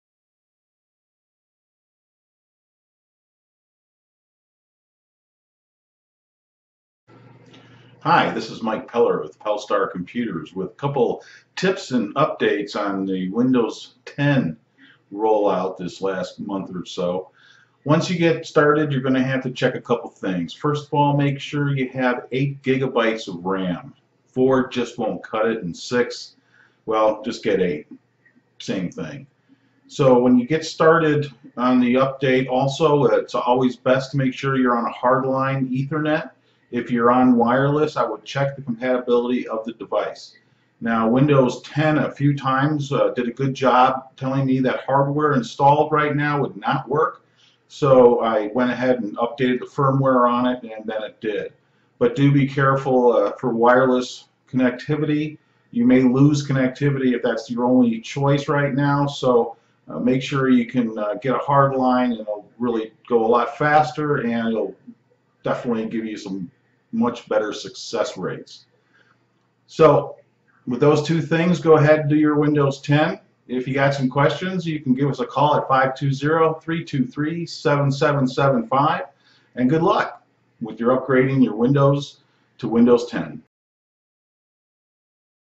Mike Windows 10 update tips number 1
Mike Peller with Pelstar computers gives his Windows 10 upgrade tips number 1
Make sure you have at least 8 GB of RAM and best to do update with Ethernet connect.  If you use wireless check compatibility before upgrading